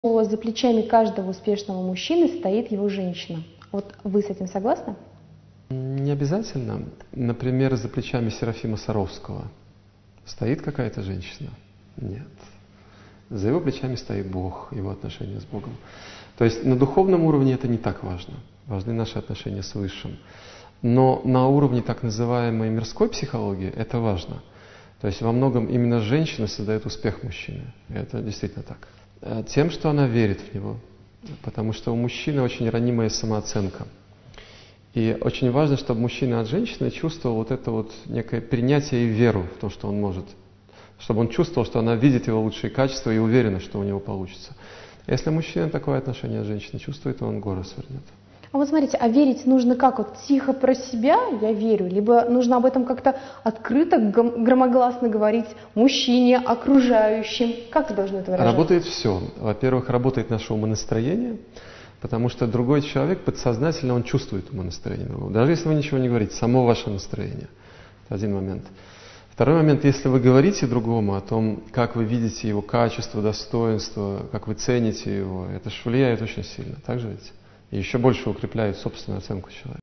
Как успех мужчины зависит от женщины. Олег Гадецкий
2. - Также посетите сайт проекта Психология третьего тысячелетия

За каждым успешным мужчиной стоит женщина.
Она оказывает непосредственное влияние на его судьбу и профессиональную сферу. Как должна вести себя женщина,чтобы ее мужчина стал сильным, уверенным и успешным, рассказывает Олег Гадецкий.

Видео фрагмент из интервью с Олегом Гадецким, «Что движет женщиной и что заставляет мужчину достигать целей». (ГТРК Чувашия, Чебоксары, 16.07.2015 г.)